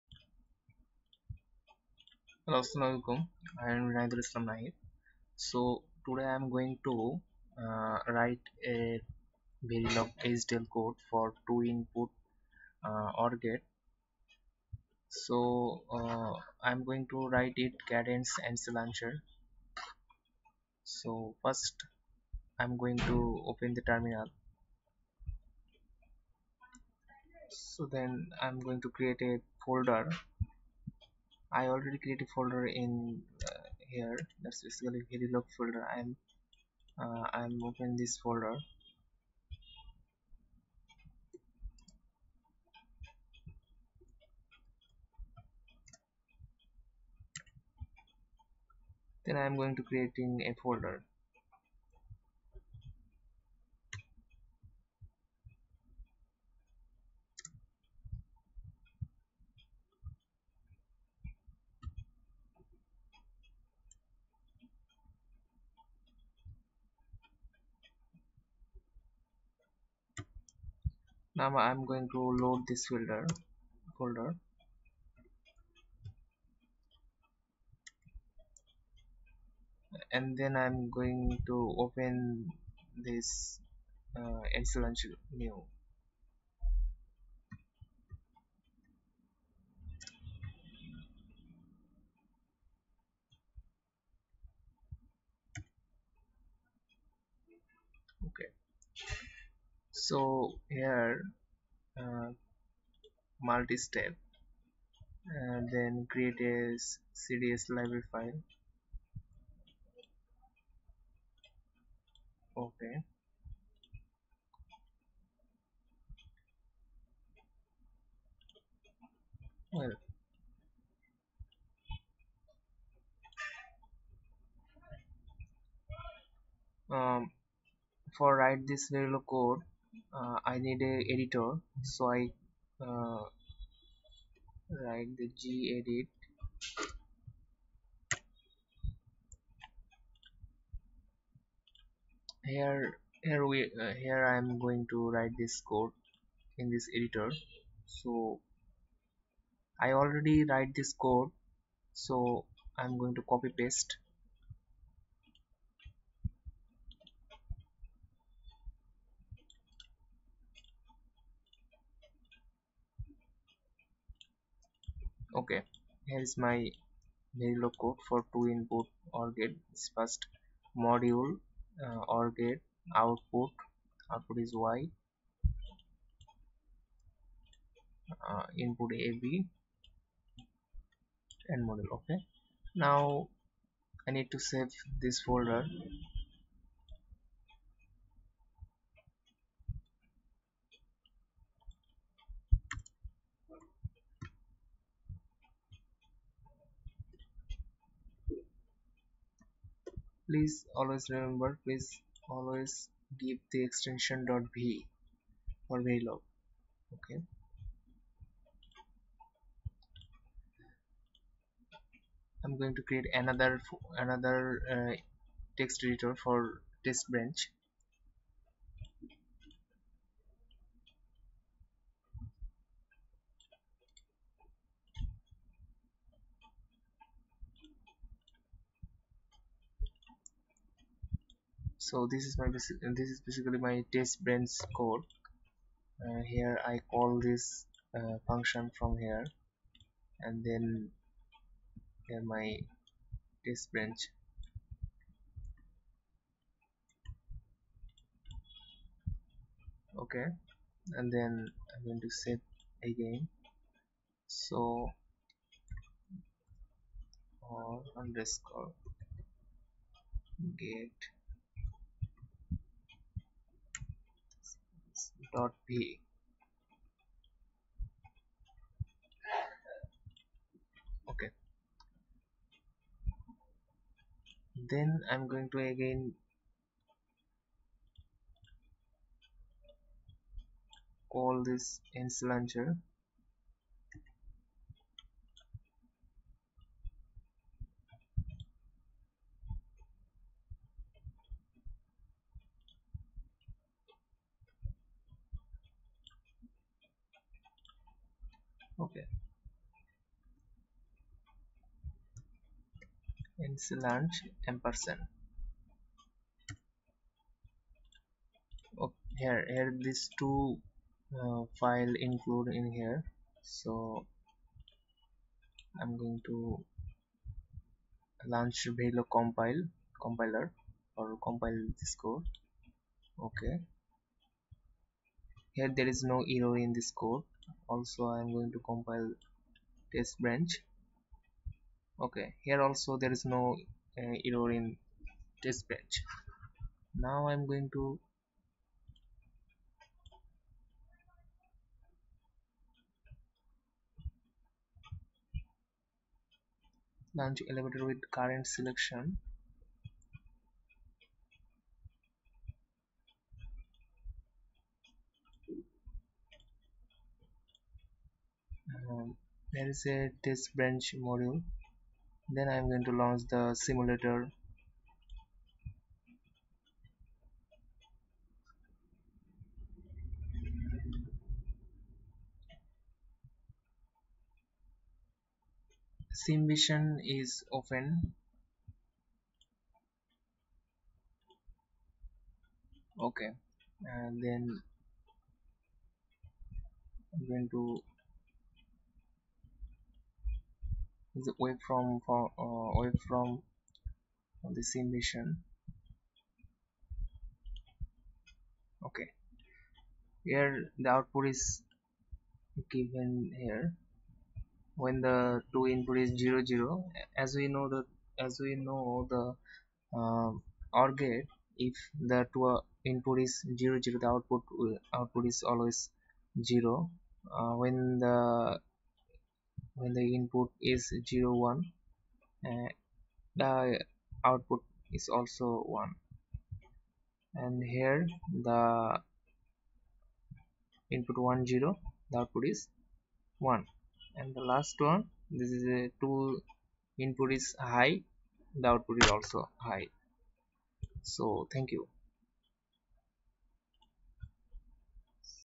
Two input OR Gate Verilog HDL Gate Level Modeling in Cadence NCLaunch
Steps of functional design and verification using Verilog HDL in nclaunch of cadence have been demonstrated in short.

NCLaunch is a graphical user interface that helps you to manage large design projects and lets you configure and launch your Cadence simulation tools. NC Launch is integrated into the Cadence Interleaved Native Compiled Architecture (INCA) and is a component of the Affirma™ SimVision analysis environment